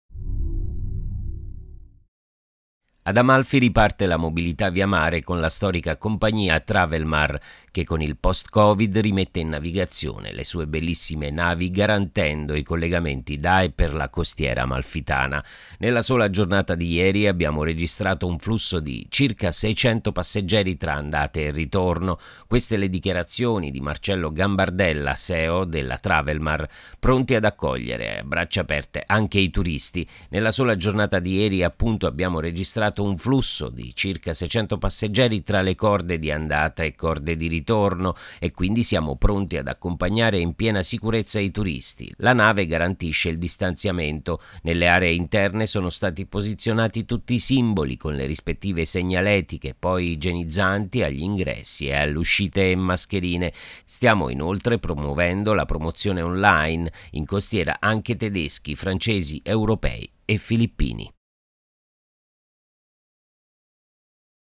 Amalfi, TravelMar riparte dal porto.
Riparte la mobilità via mare con la storica compagnia Travelmasr che con il post covid rimette in navigazione le sue bellissime navi garantendo i collegamenti da e per la Costiera Amalfitana.